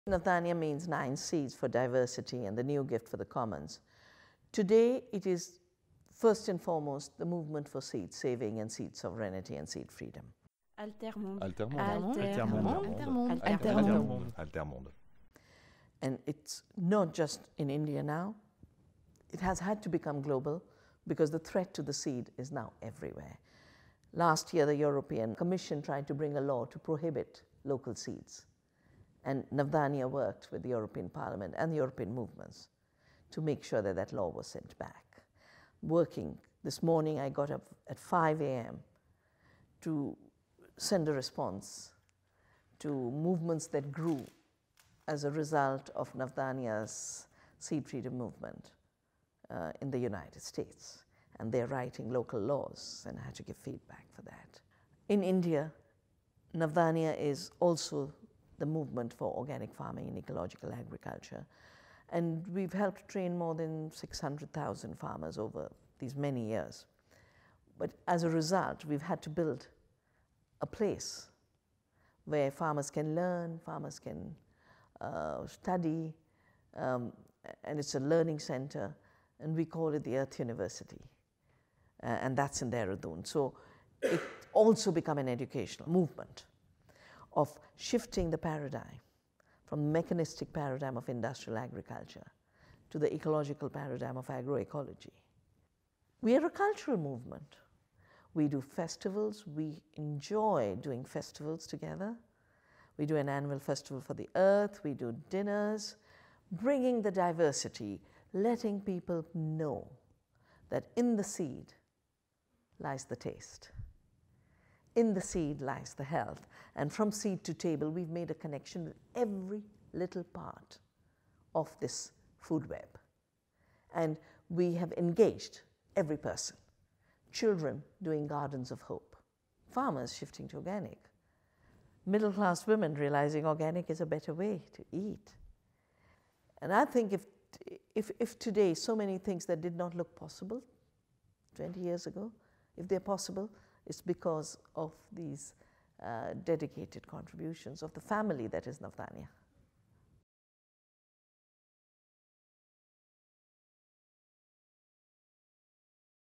[Altermondes] « Navdanya se bat pour la sauvegarde des semences locales »
Vandana Shiva, écologiste indienne, illustre les volets d'action de son mouvement, Navdanya